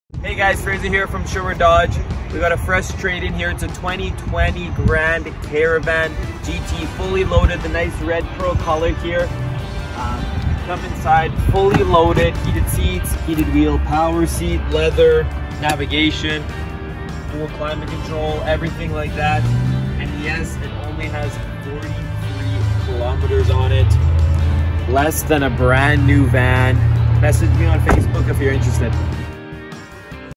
2020 Dodge Grand Caravan GT - Stock # P4943 | Sherwood Dodge
Check out this 2020 Dodge Grand Caravan GT available at Sherwood Dodge! It only has 44 KM!!

Stock # P4943

Sherwood Dodge
230 Provincial Avenue
Sherwood Park, AB
T8H 0E1
Canada